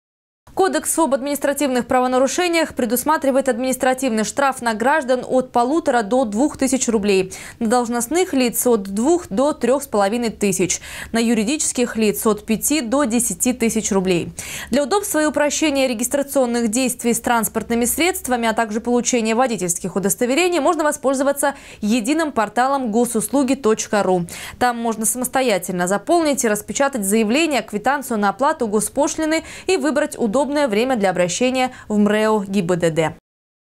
МРЭО ГИБДД Сочи призывает не тянуть с регистрацией. Новости Сочи Эфкате
МРЭО ГИБДД (по обслуживанию г. Сочи и Туапсинского района) напоминает гражданам об ответственности за нарушение сроков регистрации автомототранспортных средств, предусмотренной статьей 19.22 Кодекса об административных правонарушениях Российской Федерации.
Сочи. Новости.
Телеканал Эфкате.